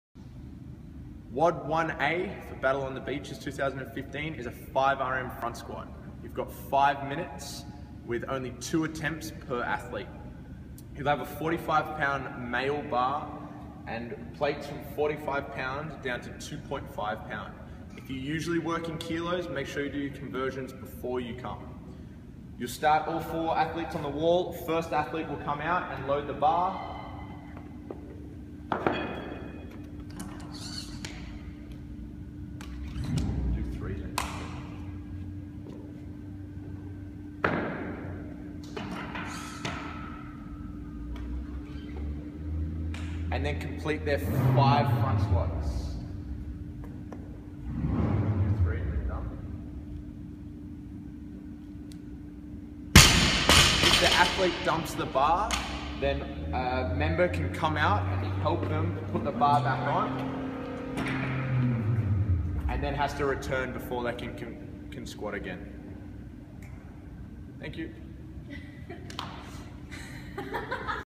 WOD 1 (a) Battle on the Beaches Games day
Athletes will have 5 minutes to find a 5rm front squat with only 2 attempts per athlete. All teams members must stay at the wall whilst 1 team member loads and completes reps. It is only when the athlete drops the weight that another athlete can come and help re-rack the bar. Teams will have a 45# male bar, and pound (Lbs) plates 45/35/25/15/10/5/2.5 for each side. Athlete will have 1 minute rest before 1(b) begins.